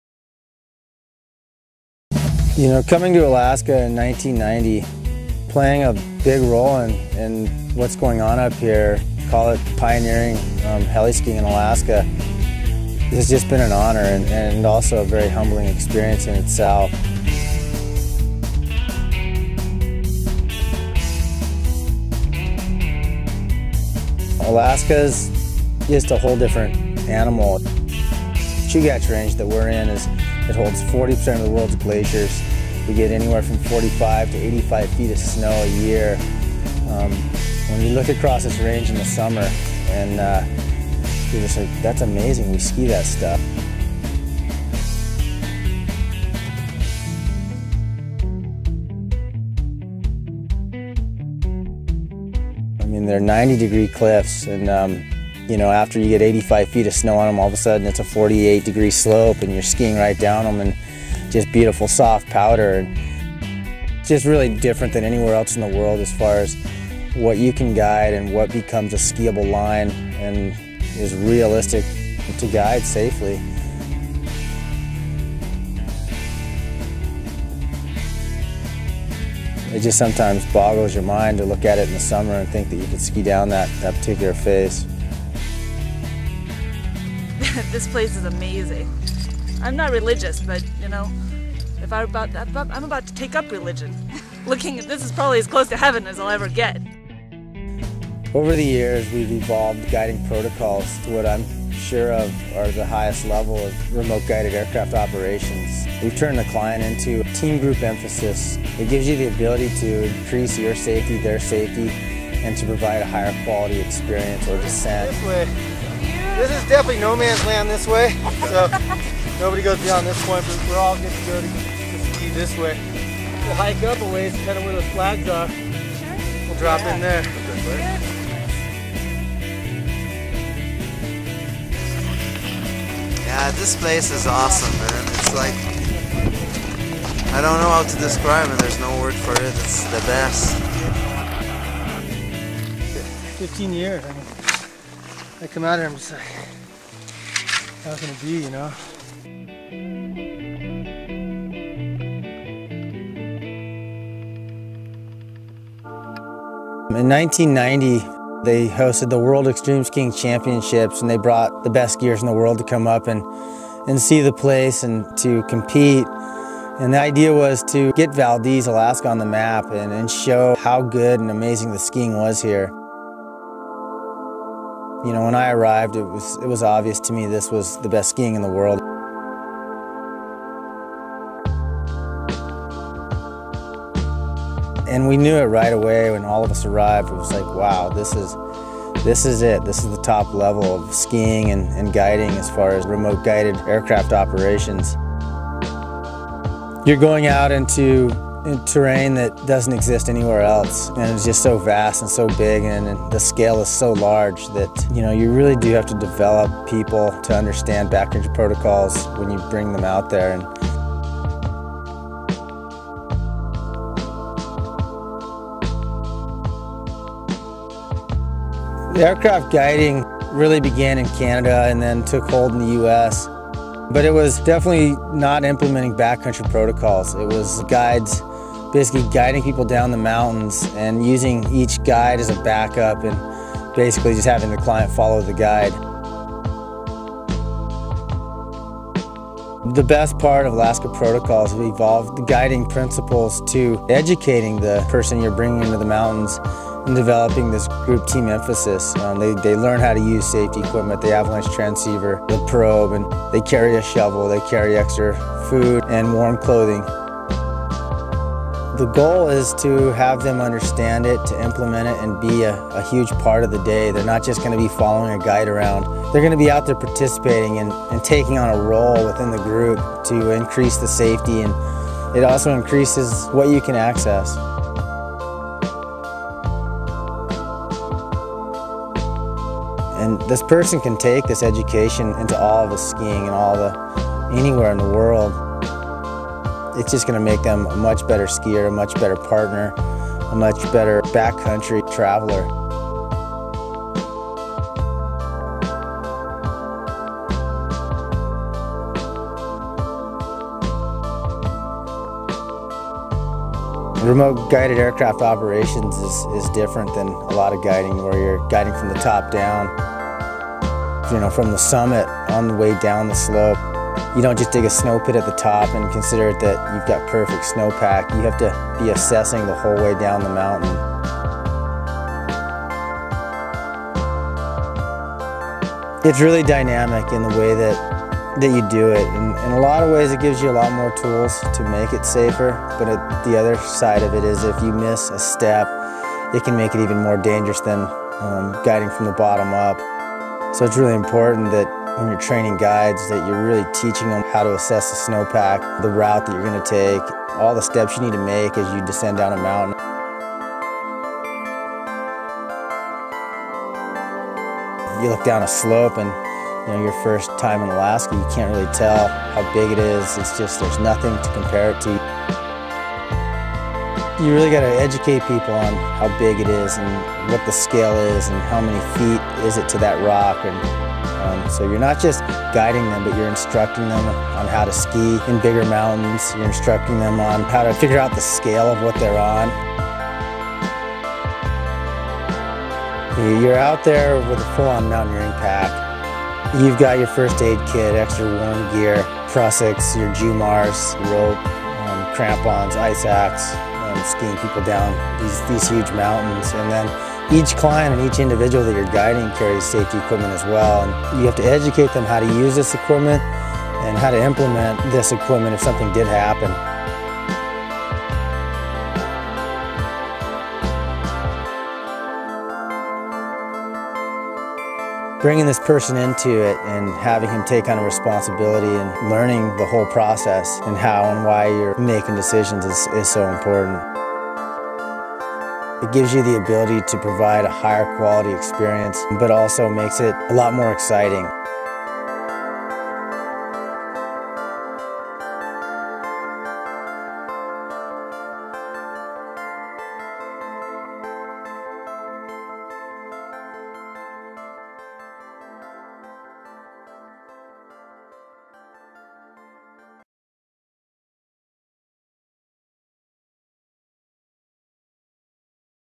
Alaska Helicopter Skiing with H2O Guides
A look at what its like to pioneer the Chugach Range of Valdez, Alaska.